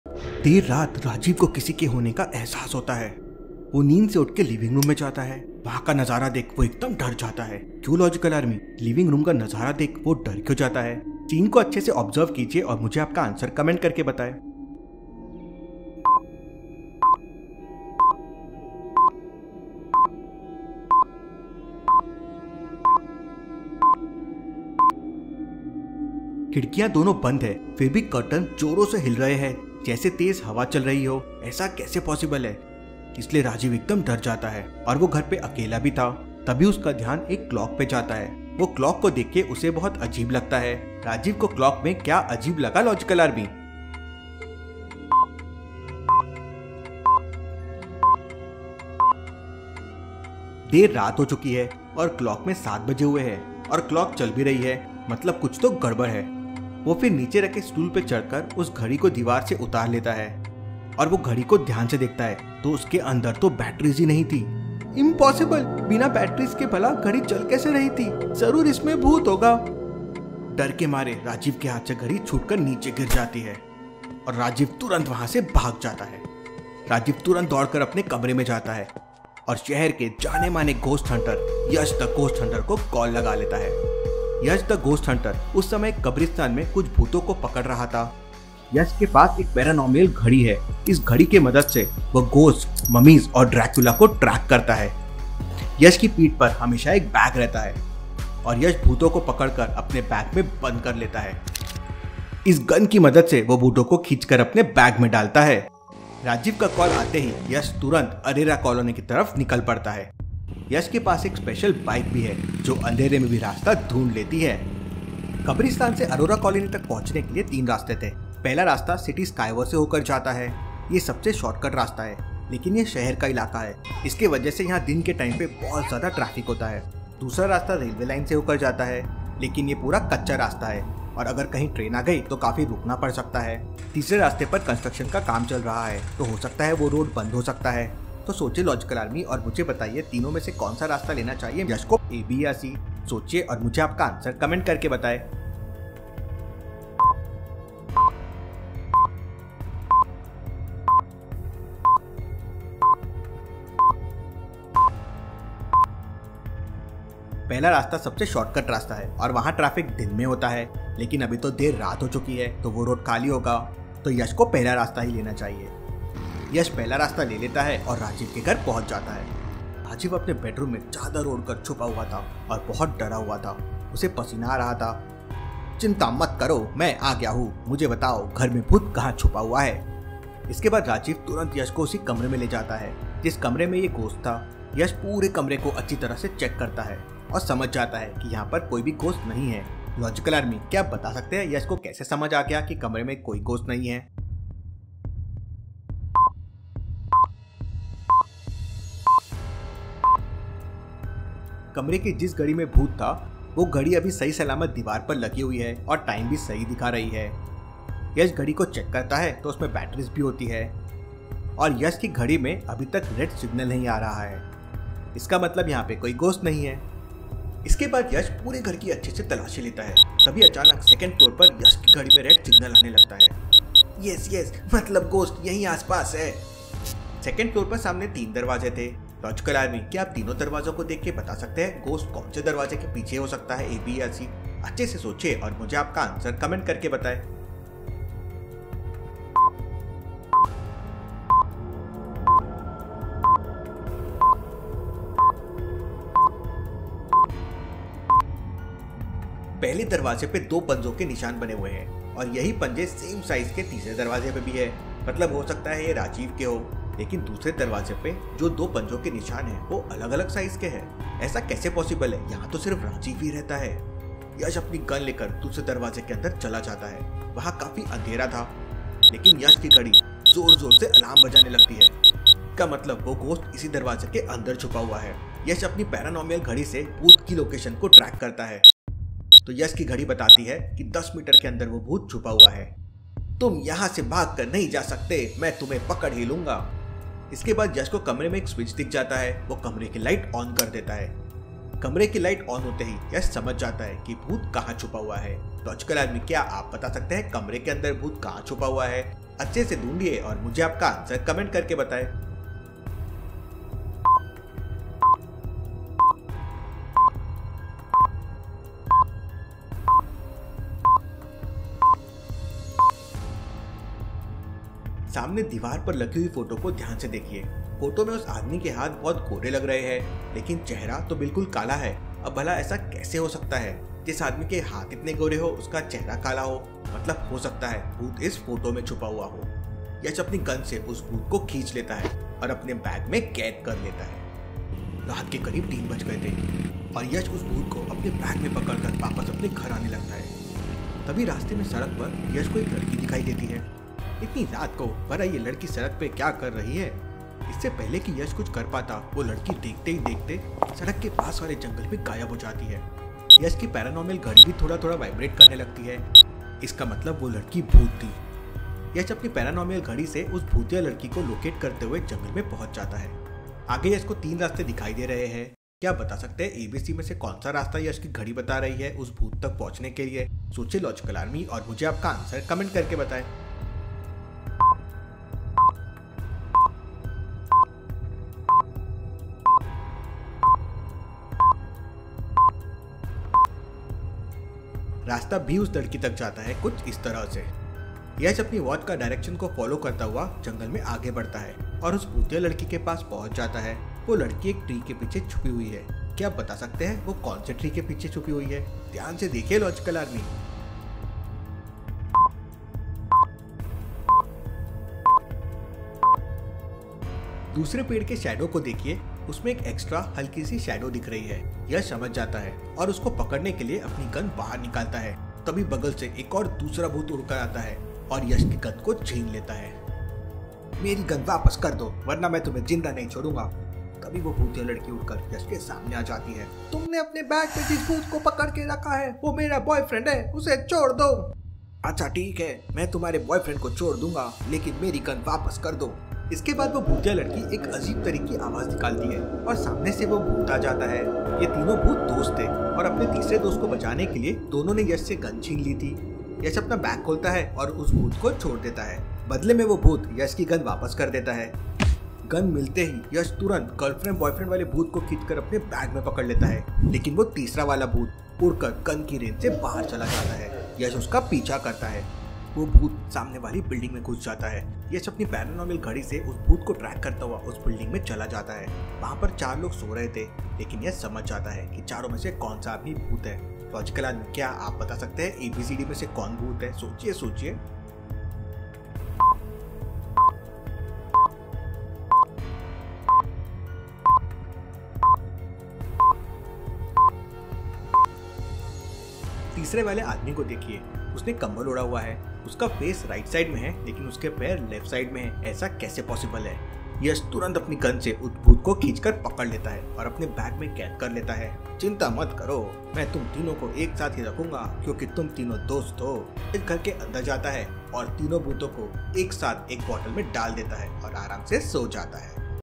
Yash the ghost hunter ( Episode 1 ) | Hindi Paheli | Paheliyan | Riddles in Hindi | Mind Your Logic
Yash is the ghost hunter. People call him to catch ghost, mummies, drakula in their houses, farms, schools, colleges and many more places.

He has a paranormal watch in this hand which helps him to track the ghost.
He solves many riddles in hindi, paheliyan to catch the ghost.

He also carries a bag with him in which he keeps the ghosts. These ghosts are very scary and dangerous.
Join the Yash the ghost hunter in his mission to catch these scary ghosts, mummies and drakula.
In this journey you will find many tricky paheli, murder mysteries and paheliyan, lets see if you can solve those.